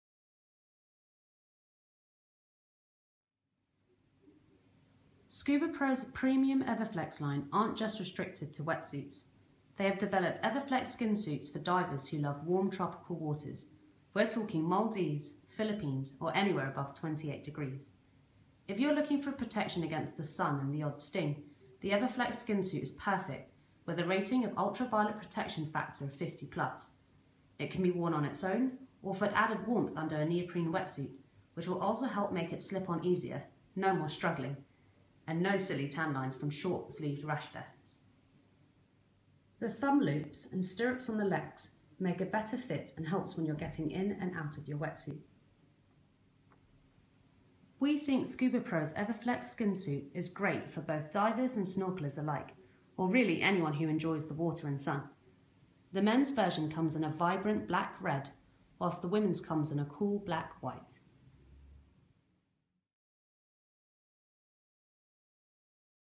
Scubapro Everflex Full Skin Suit
Scubapro's Premium Everflex Line also includes full Skin Suits - perfect for tropical water divers and snorkelers!

The Scubapro Everflex Skin Suit is perfect for warmer waters where you only really need some sun and stinger protection. So perfect for the Maldives, Phillipines and anywhere else where diving is above 28 degrees. The suit is available in male and female cuts.